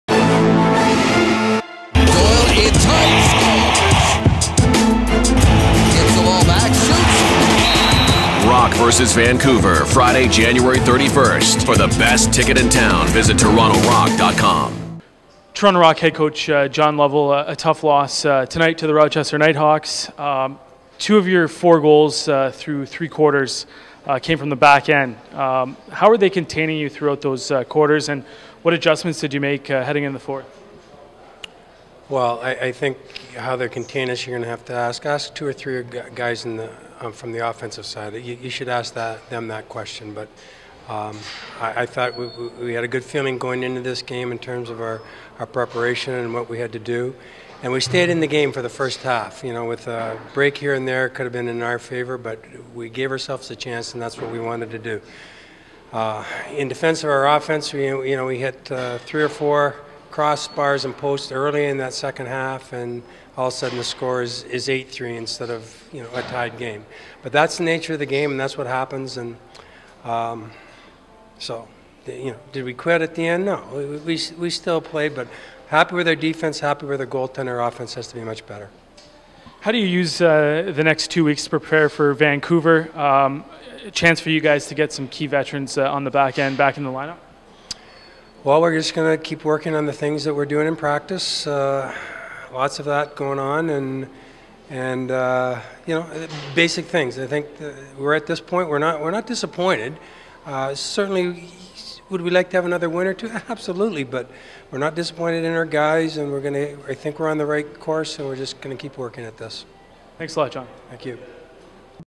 Toronto Rock Head Coach John Lovell Post Game Interview January 18th, 2014
Toronto Rock Head Coach John Lovell comments after losing 12-8 to the Rochester Knighthawks in Week 4.